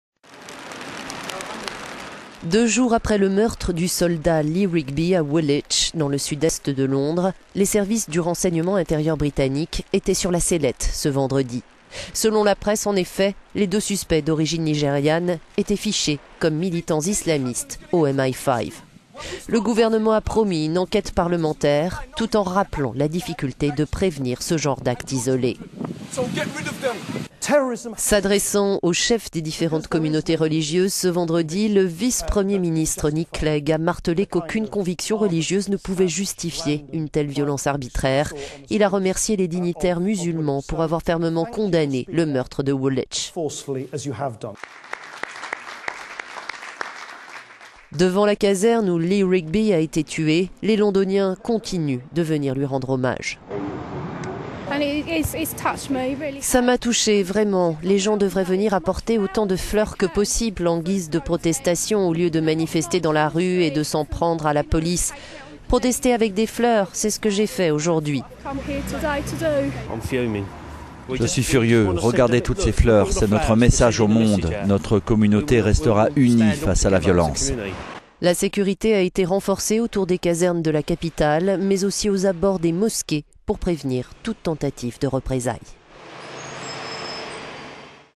Soldat tué à Londres : le MI5 sur la sellette
Deux jours après le meurtre du soldat Lee Rigby à Woolwich, dans le sud-est de Londres, les services du renseignement intérieur britannique étaient sur la sellette ce vendredi.

Selon la presse en effet, les deux suspects d'origine nigériane étaient fichés comme militants islamistes au MI5.

Le gouvernement a promis une enquête parlementaire, tout en rappelant la difficulté de prévenir ce genre d'acte isolé.

S'adressant aux chefs des différentes communautés religieuses ce vendredi, le vice-premier ministre, Nick Clegg, a martelé qu'aucune conviction religieuse ne pouvait justifier une telle violence arbitraire. Il a remercié les dignitaires musulmans pour avoir fermement condamné le meurtre de Woolwich.

Devant la caserne où Lee Rigby a été tué, les Londoniens continuent de lui rendre hommage en déposant des bouquets de fleurs, des ballons et des messages.

La sécurité a été renforcée autour des casernes de la capitale mais aussi aux abords des mosquées, pour prévenir toute tentative de représailles.